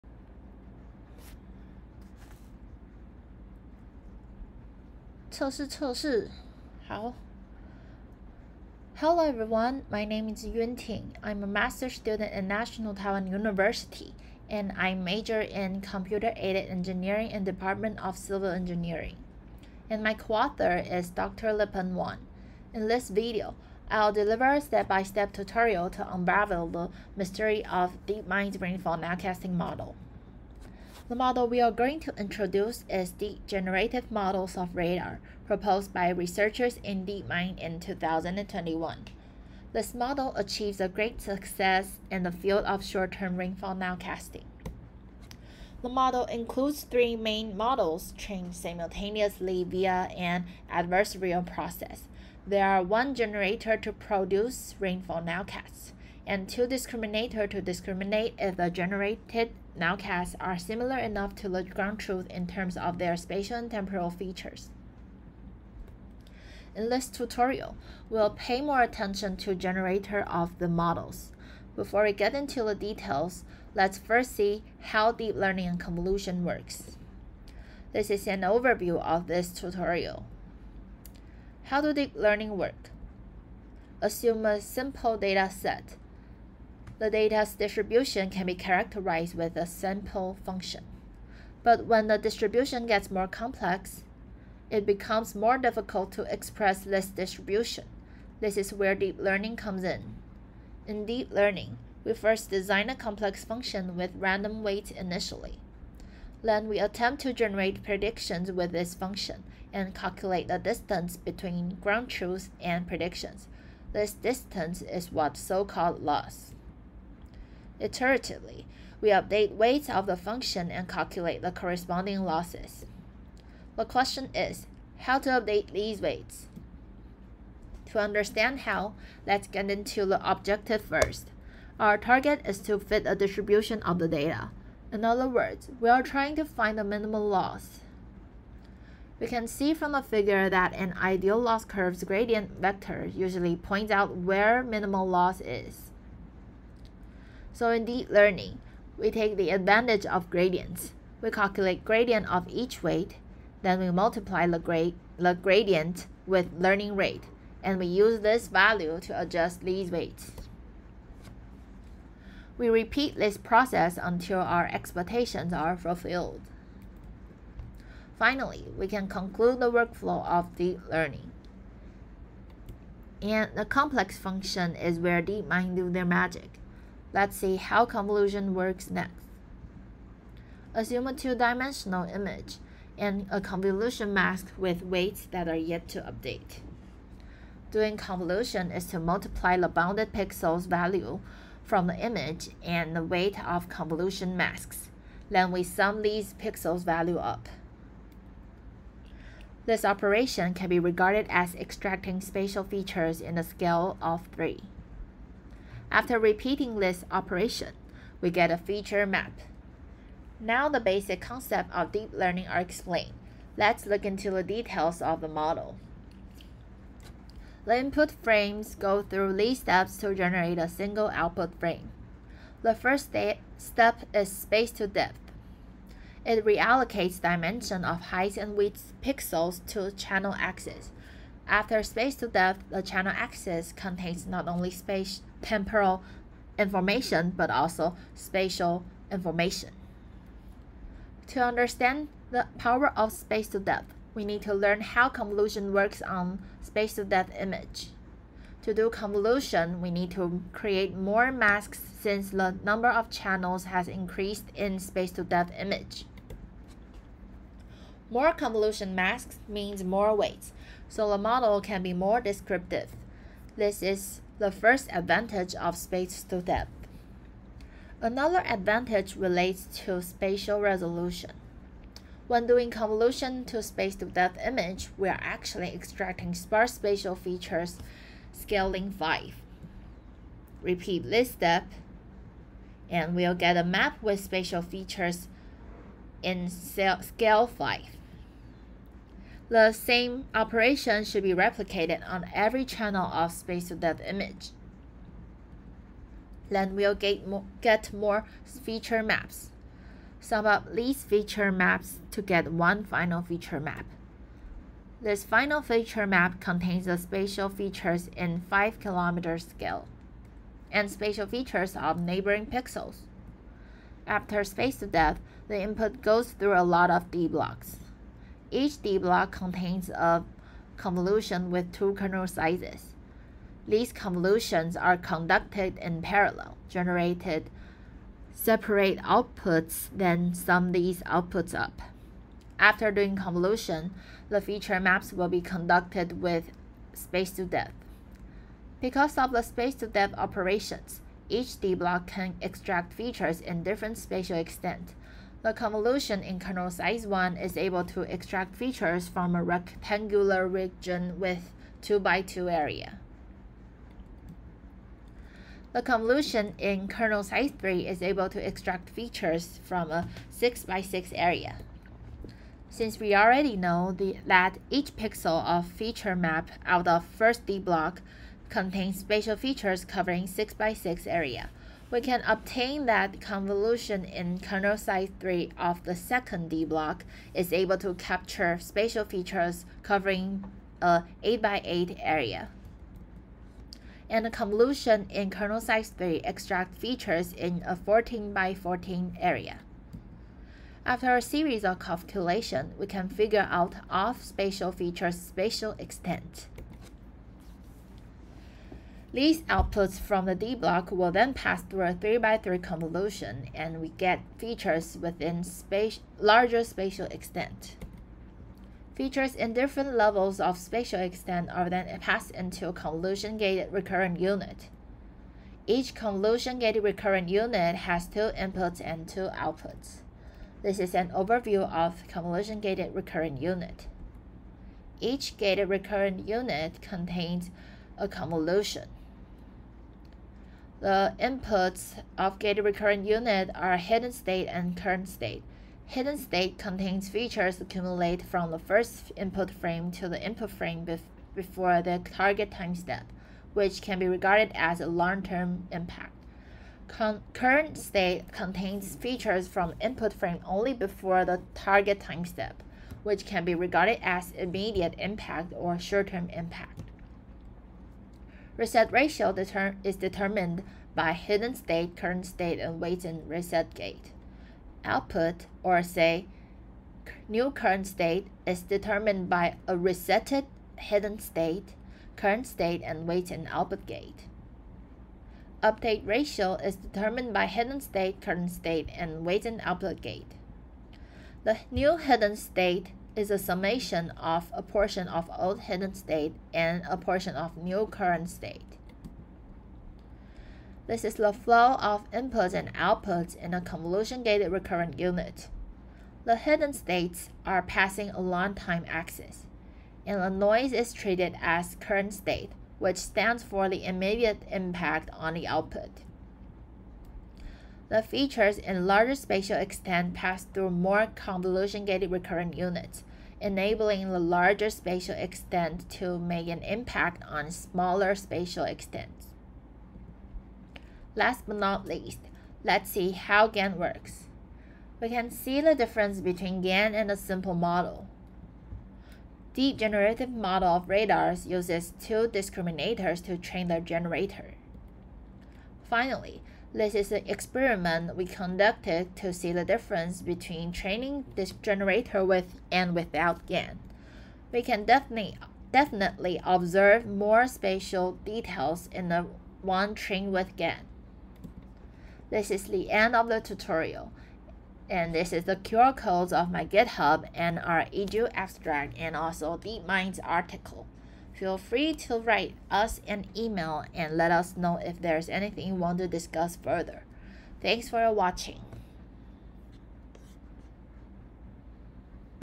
DGMR: Skilful precipitation nowcasting using deep generative models of radar
DGMR is a deep-learning-based rainfall nowcasting model proposed by DeepMind. It uses space to depth as downsampling method and adversarial training to preserve more spatial details in their prediction.

In this video, a brief intro of deep learning and convolution is given. Details of DGMR's generator are specified, especially how the model deals with multi-scale spatial features.

EGU22 abstract
Unraveling the mystery of DeepMind’s rainfall nowcasting: a step-by-step tutorial for hydrologists

My reproduction of DGMR on Github (weights are yet to update)

Skilful precipitation nowcasting using deep generative models of radar